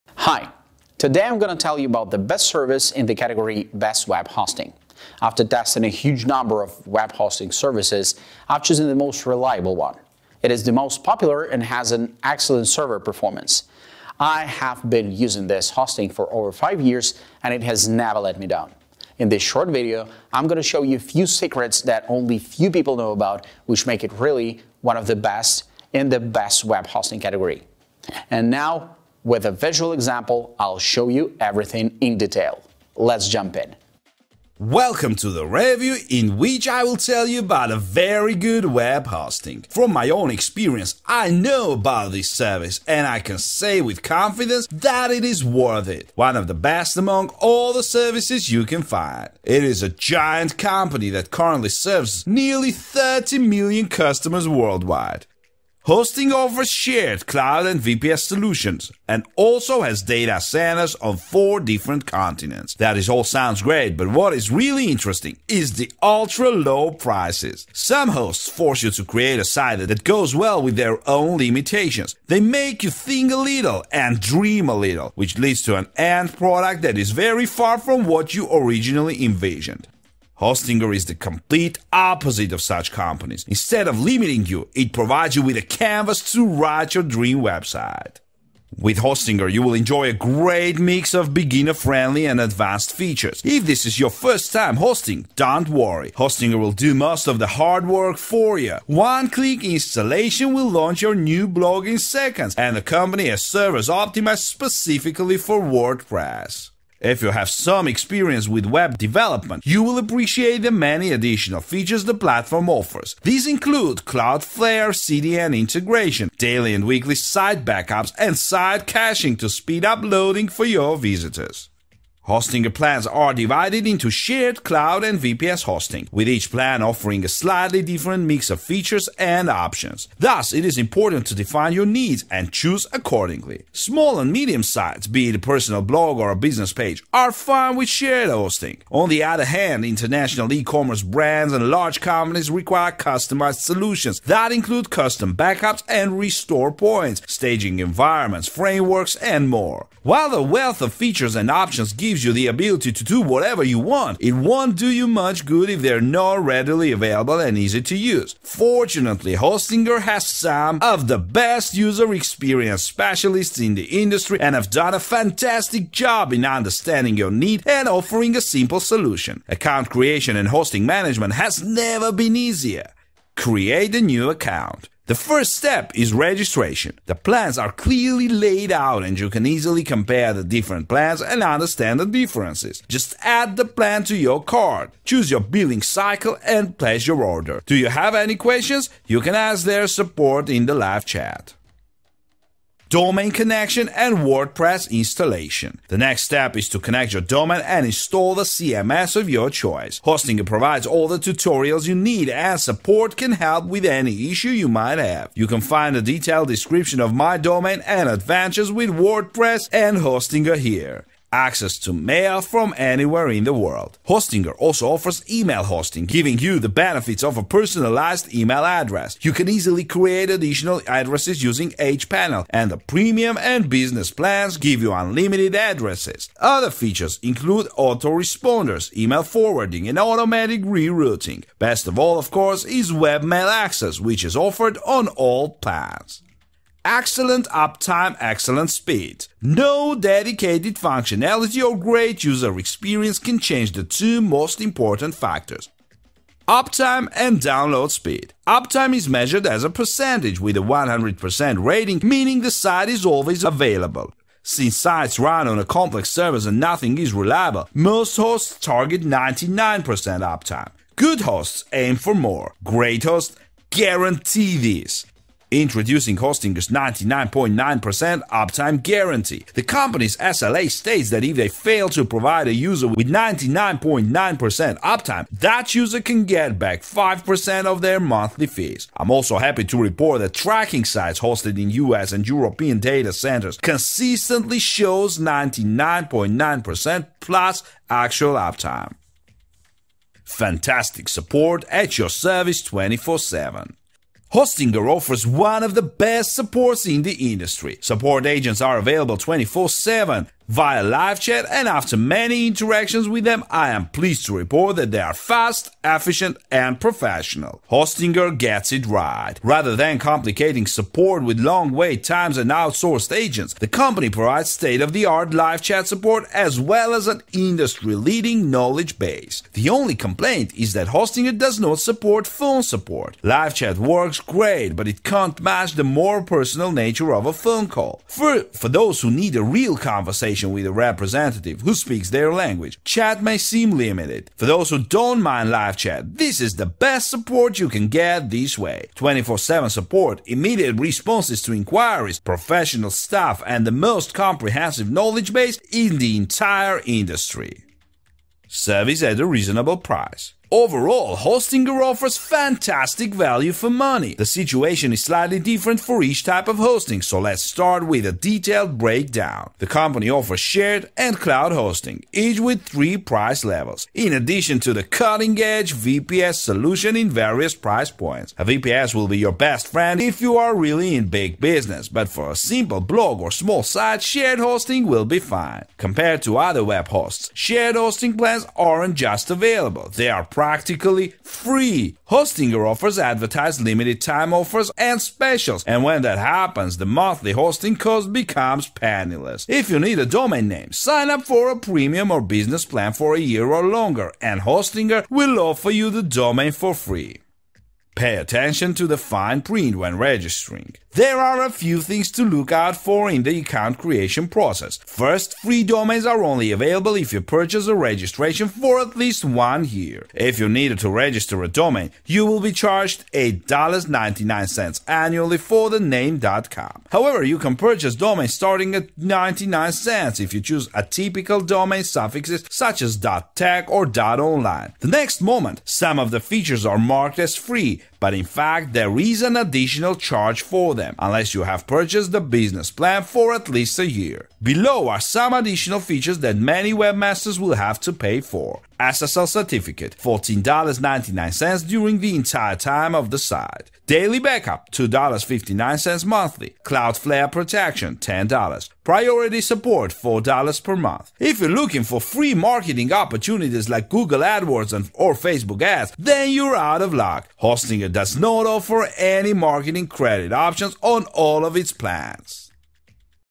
Best Web Hosting That Help You Make Money
00:01 - Intro
00:32 - Quality Web Hosting
01:26 - Best Web Hosting
03:45 - Hostinger Review
05:01 - How to Choose Web Hosting
07:44 - Outro
Welcome to the review in which I will tell you about very good web hosting. From my own experience I know about this service and I can say with confidence that it is worth it. One of the best among all the services you can find.
It is a giant company that currently serves nearly 30 million customers worldwide. Hosting offers shared, cloud and VPS solutions, and also has data centers on four different continents.
This all sounds great, but what is really interesting is the ultra-low prices.
Some hosts force you to create a site that goes well with their own limitations. They make you think a little and dream a little, which leads to an end product that is very far from what you originally envisioned.
Hostinger is the complete opposite of such companies. Instead of limiting you, it provides you with a canvas to write your dream website.
With Hostinger, you will enjoy a great mix of beginner-friendly and advanced features. If this is your first time hosting, don't worry - Hostinger will do most of the hard work for you. One-click installation will launch your new blog in seconds, and the company has servers optimized specifically for WordPress.